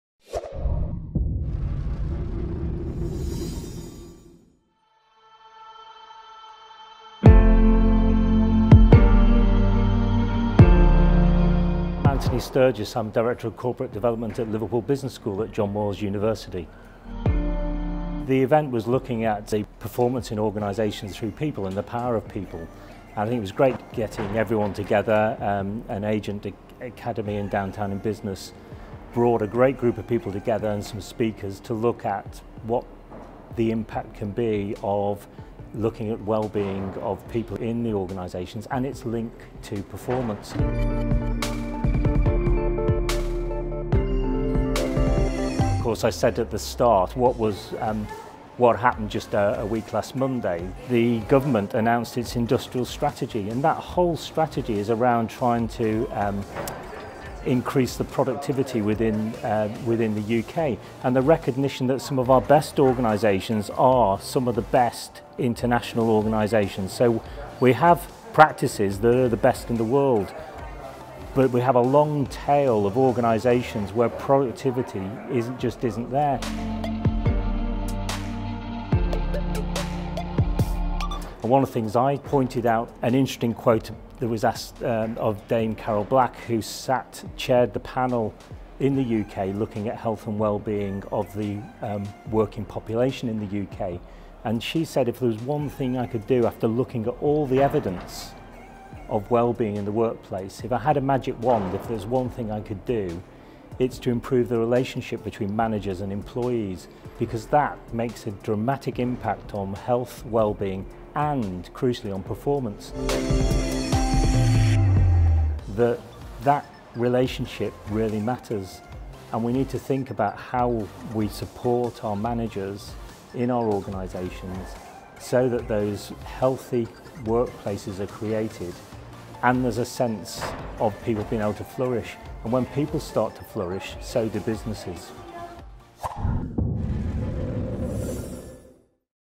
Investing In People Power // Agent Marketing and Downtown in Business
Radisson Blu Liverpool played host to the “Investing in People Power” discussion organised by Agent Academy and Downtown Liverpool in Business. Central to the discussion was the concept of leadership, together with ways in which businesses can get their best from employees to improve productivity and growth.